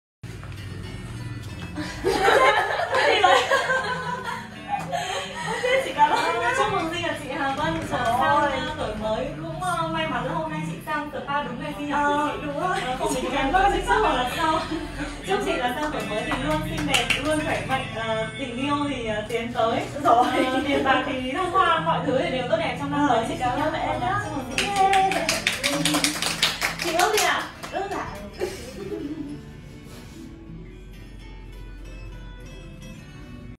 Chúc mừng sinh nhật chị Hà Vân - Mudra House
Mudra_House chúc mừng sinh nhật chị đẹp Nguyễn_Hà_Vân 😍😍😍
Đã là mẹ 2 con nhưng trông chị thật tươi trẻ, người phụ nữ hạnh phúc là người biết trân trọng chính mình trước tiên. Chúc chị sang tuổi mới luôn hạnh phúc bên gia đình nhỏ và gặt hái nhiều thành công trong sự nghiệp ❤️
Mudra House là vậy đấy, chân thành đúng nghĩa với phương châm “ ấm áp như được trở về nhà “, không chỉ các bác trung - cao tuổi, mà các cô, các chị và tất cả thành viên trong gia đình đều có thể gửi gắm nơi đây ! 🎁